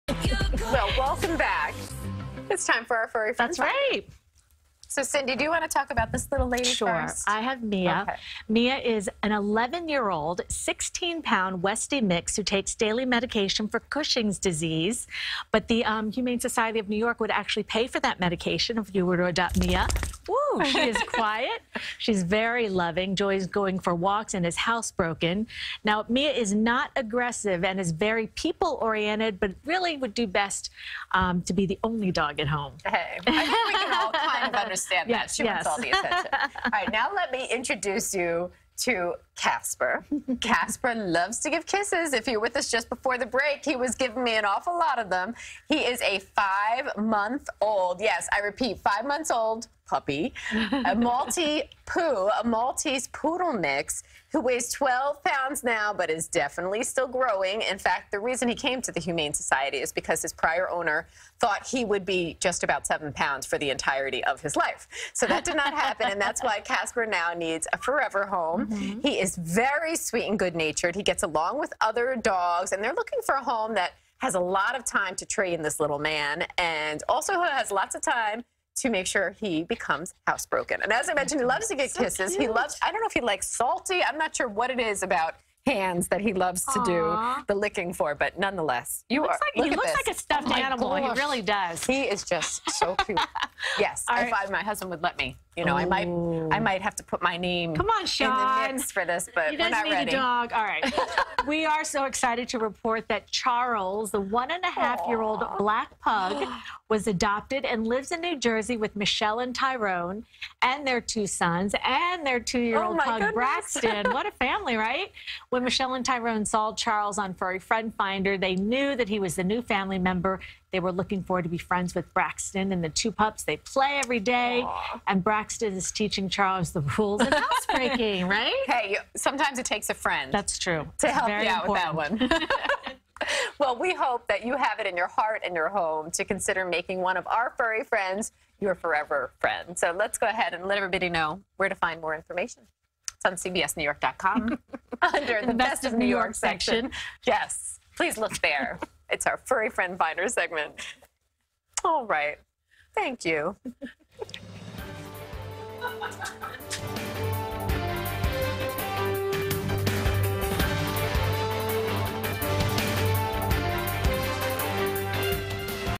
Furry Friend Finder: Mia & Casper
Mia is an 11-year-old Westie mix and Casper is a 5-month-old Maltese-Poodle mix.